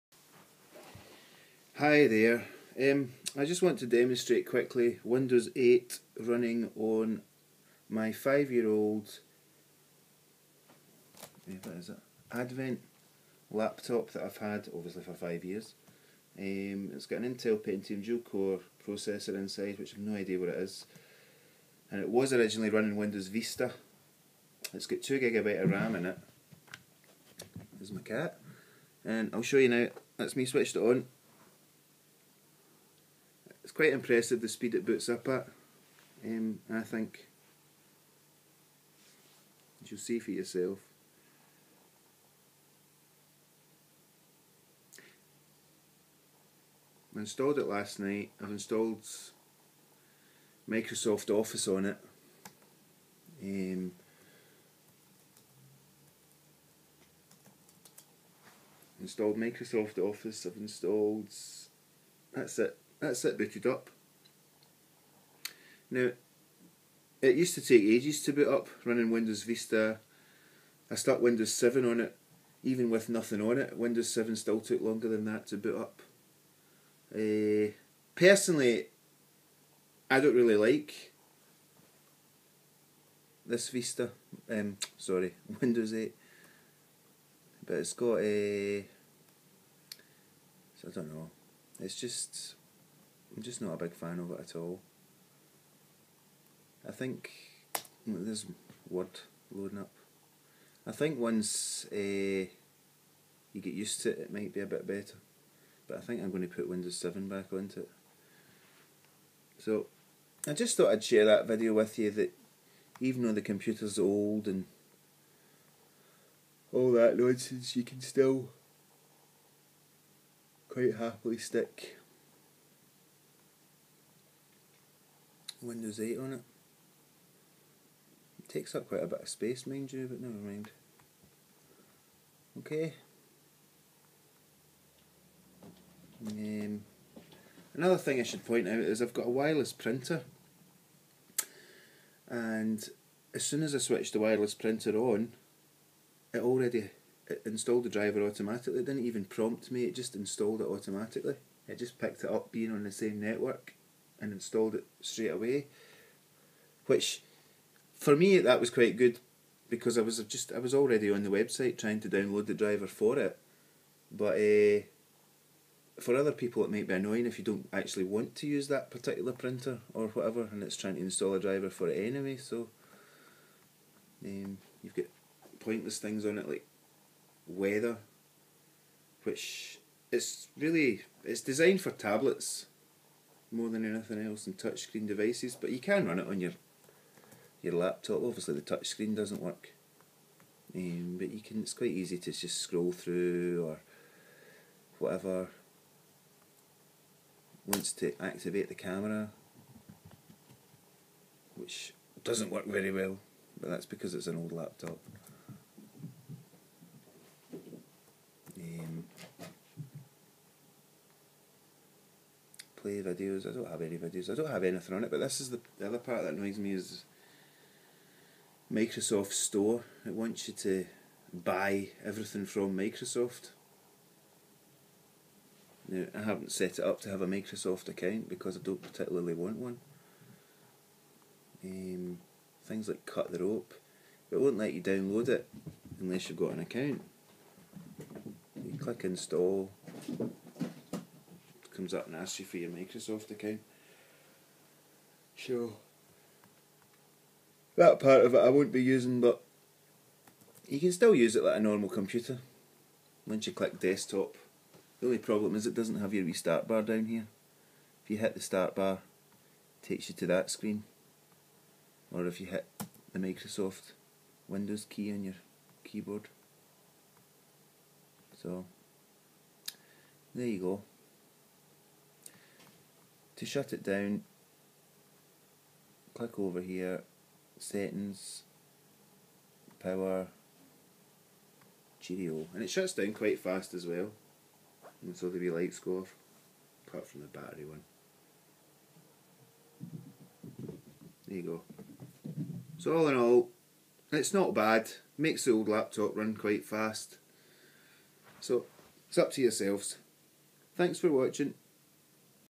Windows 8 running on old laptop
I installed windows 8 onto my 5 year old laptop, it's a duel core intel processor, with 2 gb of ram. End result was quite impressive as it made my old laptop run quite fast. Take a look and see for yourself